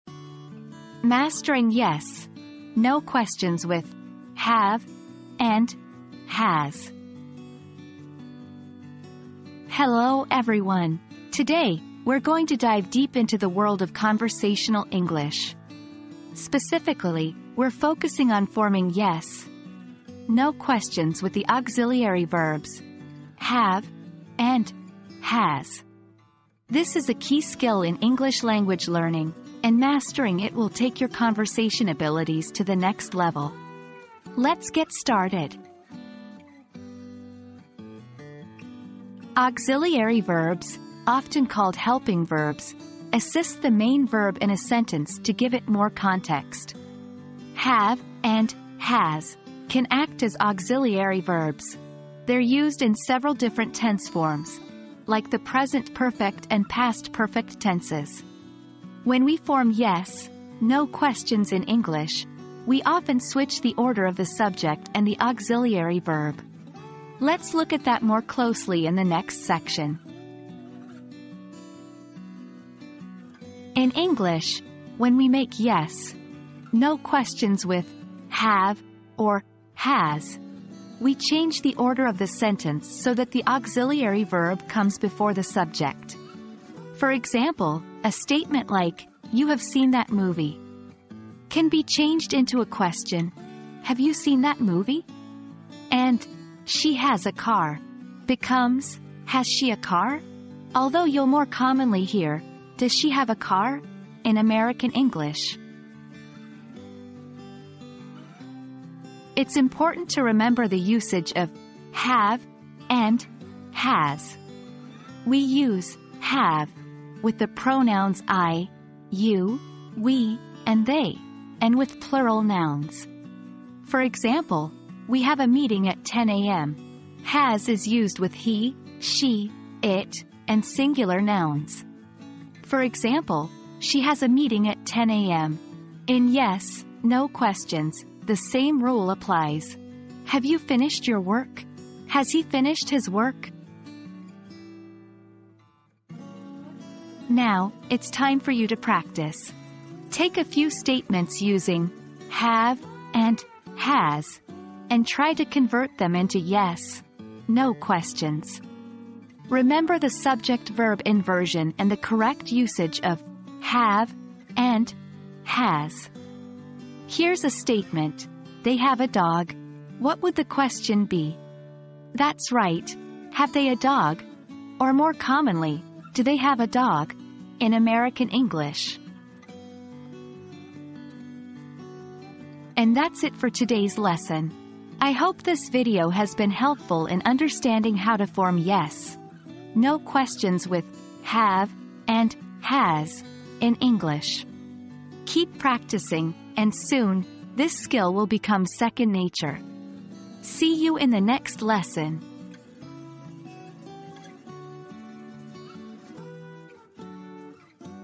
Mastering Yes/No Questions with 'Have' and 'Has'
Mastering Yes/No Questions with 'Have' and 'Has' • Dive deep into the world of conversational English and learn how to form yes/no questions with the auxiliary verbs 'have' and 'has'. This key skill will take your conversation abilities to the next level. Let's get started!

00:00 • Introduction - Mastering Yes/No Questions with 'Have' and 'Has'
00:38 • Understanding Auxiliary Verbs
01:13 • Forming Yes/No Questions with 'Have' and 'Has'
01:48 • 'Have' vs 'Has'
02:26 • Practice Makes Perfect